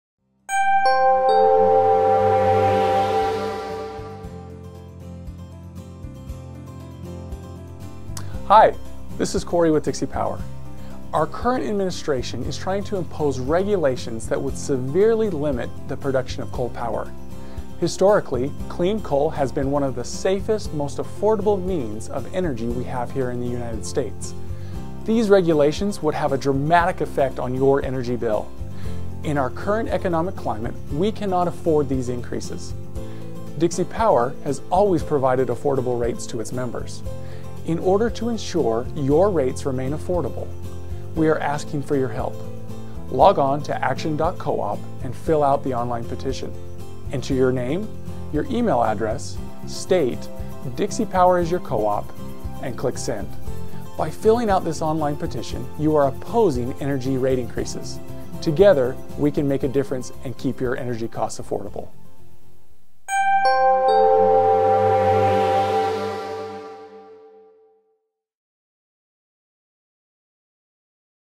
Support your local electrical coop at action.coop
Support your local electrical coop and get your Message to the Environmental Protection Agency (EPA)
The Administration has repeatedly said that it supports an "all-of-the-above" energy strategy. Electric co-ops have pursued "all-of-the-above" by spending billions on innovative, clean and efficient technologies.

But the EPA's proposal to regulate greenhouse gas emissions at new power plants seems to be an "all-but-one" approach.